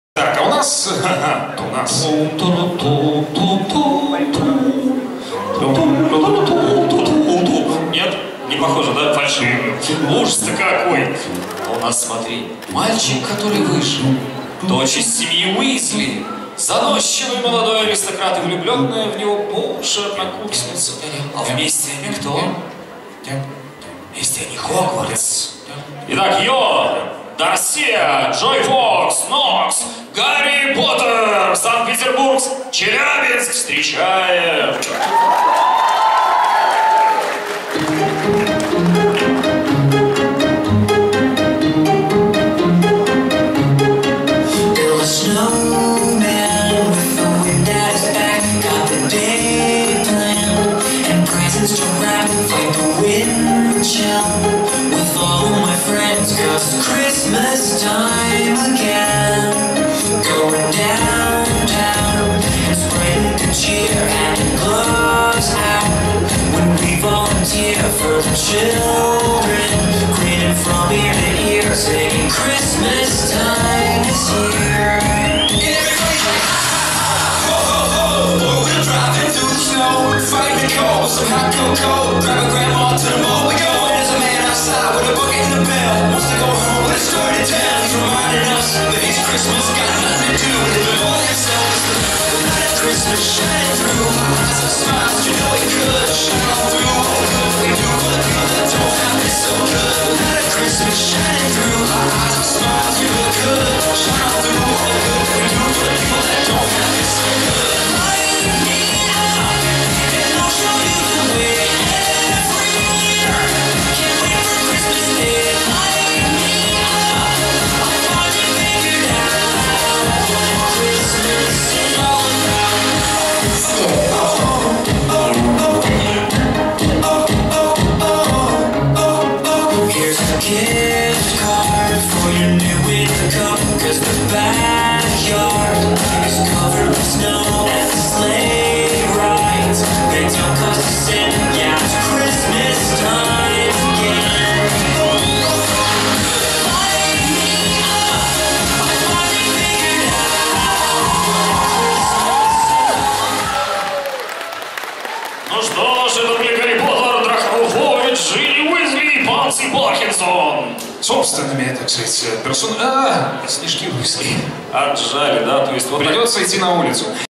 Harry Potter Defile - Harry Potter, Draco Malfoy, Ginny Weasley, Pansy Parkinson
cosplay defile West
Cosplayers: Yoh, Darcia, Joy~Fox, Noks
FreeTime Fest 2018 31/03/2018
Video by RockHide